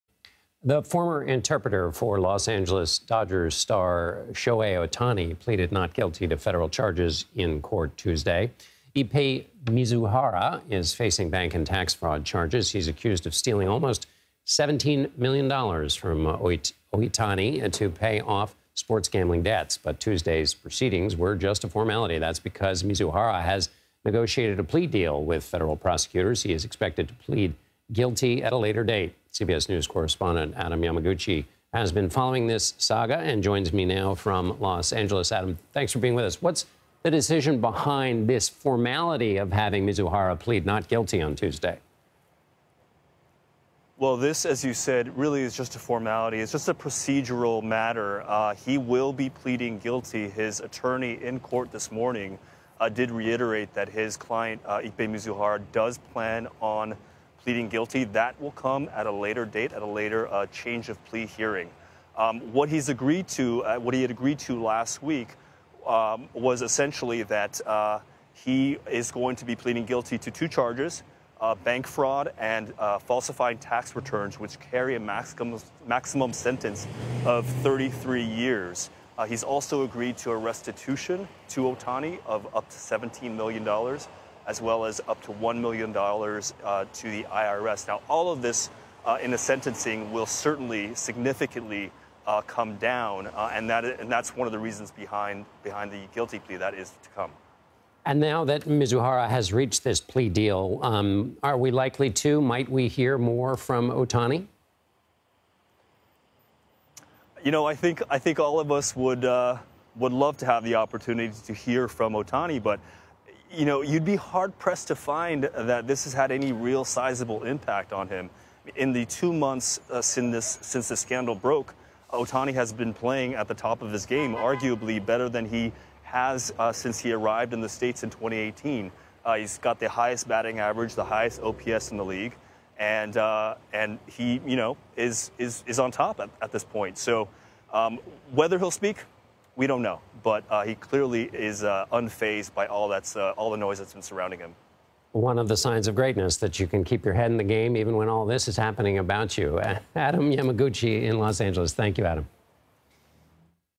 What we know about the plea deal for Shohei Ohtani's ex-interpreter
The former interpreter for Los Angeles Dodgers star Shohei Ohtani pleaded not guilty to federal charges in court Tuesday as a formality. Ippei Mizuhara has negotiated a plea deal with federal prosecutors and is expected to plead guilty eventually. CBS News correspondent Adam Yamaguchi has more.